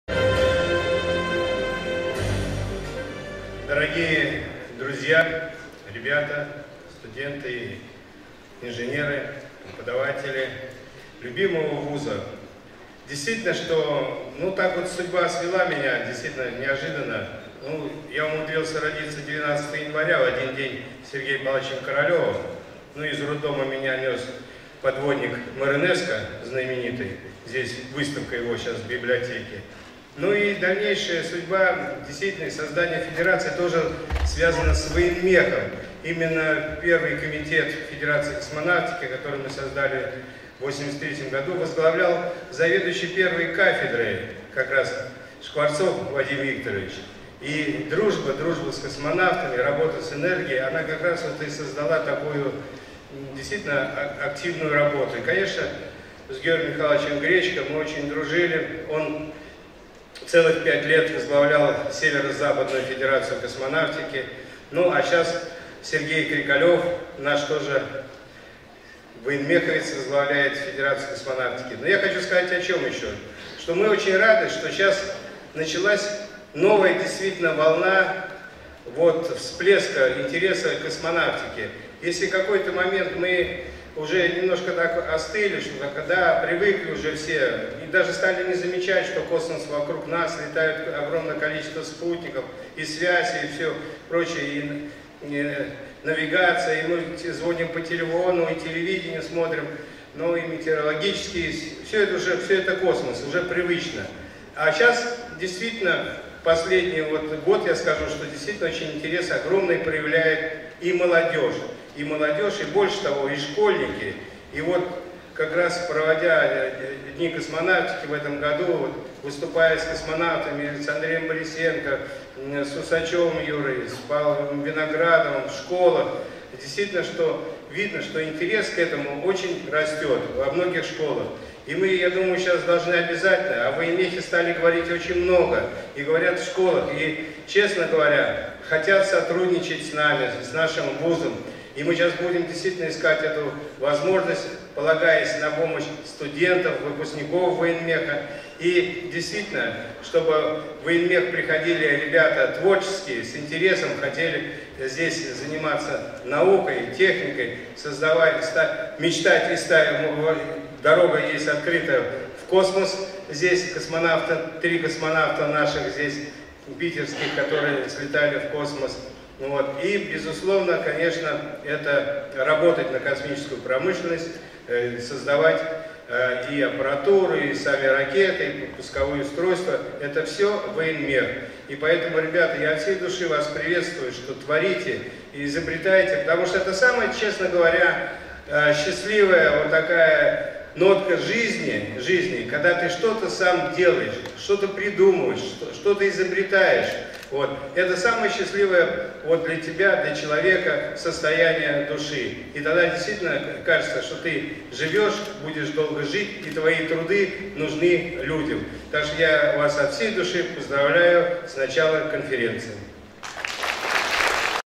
00017
Х научно-техническая конференция «Молодежь. Техника. Космос»
Конференция посвящена памяти выдающегося выпускника «Военмеха», летчика-космонавта, дважды-героя Советского Союза – Георгию Михайловичу Гречко.
Пленарное заседание